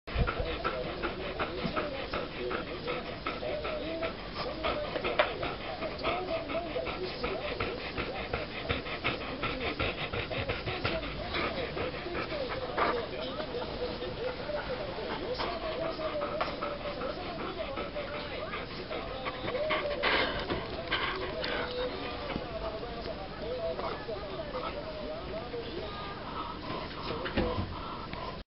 おねだりマーキュン
１３歳と１か月のマーキュン。ず～っと寝ているかと思えば、突然、私のところへやってきて「おねだり」。
年よりは食べることだけが楽しみ？？
貧血のため、呼吸が荒いので、いつもこんな感じです。